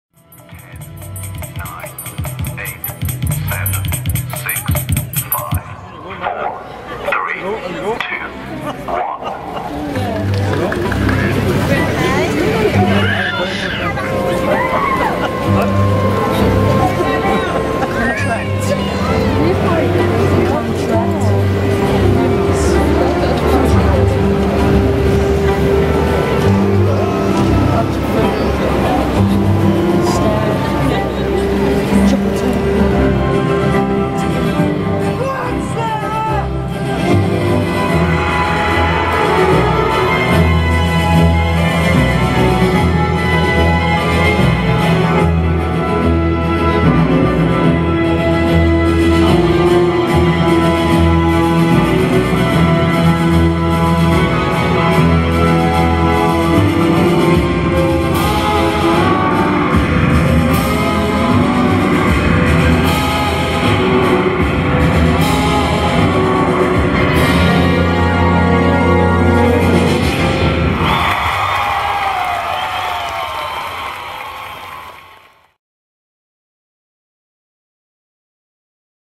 Mass Bolero Dance in Nottingham
Mass Bolero dance mob in The Market Square Nottingham
12th July 2014